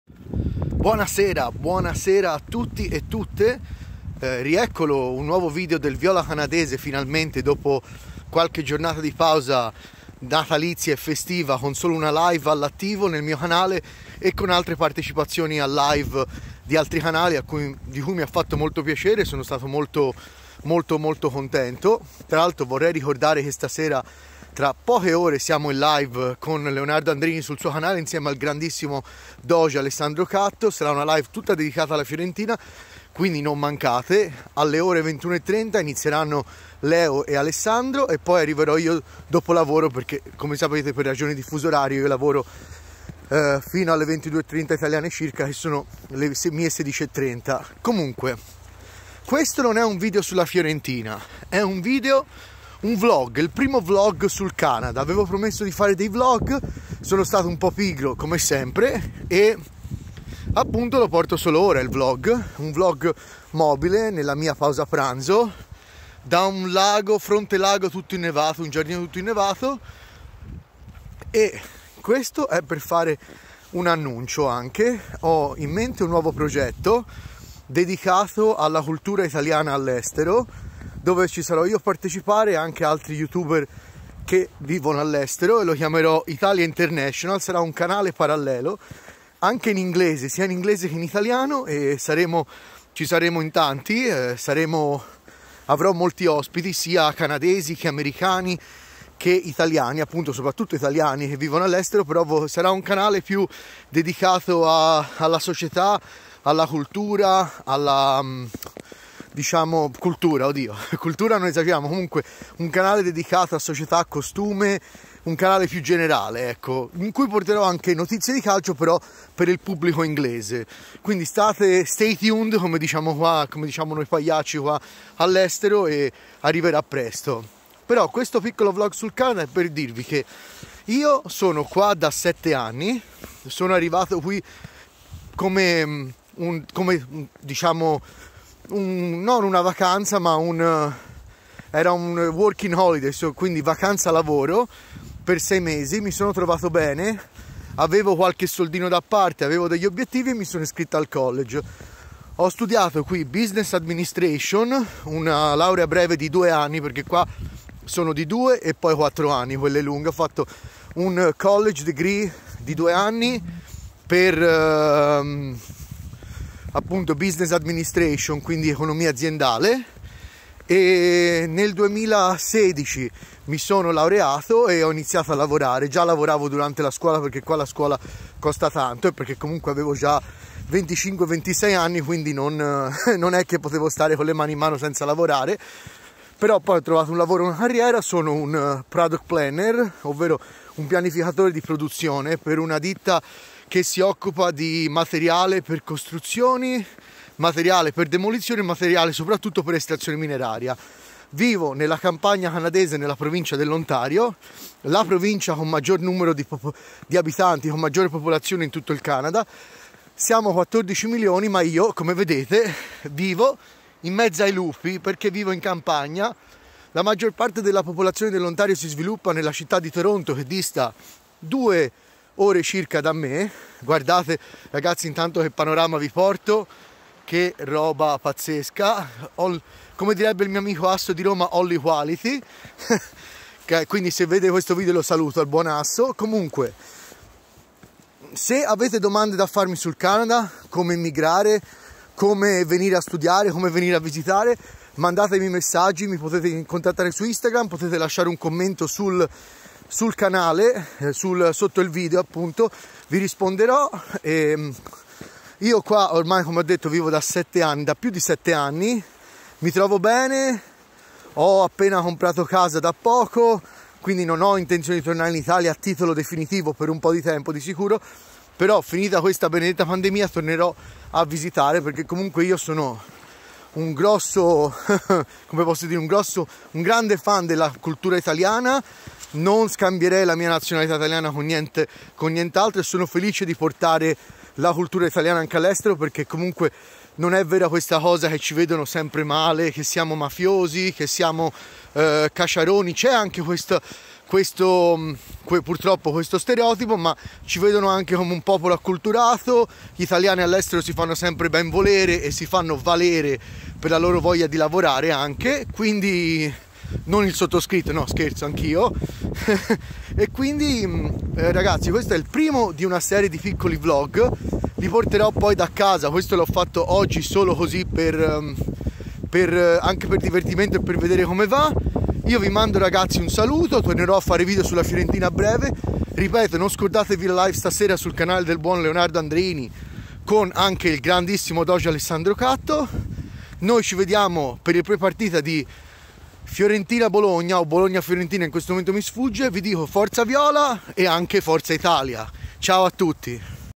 Canada Vlog #1
Primo Vlog dedicato alla vita in Canada di questo Fiorentino pazzo del Viola Canadese!

Un video un po' goliardico, ma che fa da introduzione ad una nuova rubrica.

inoltre doppio annuncio nel video di oggi!

SFV!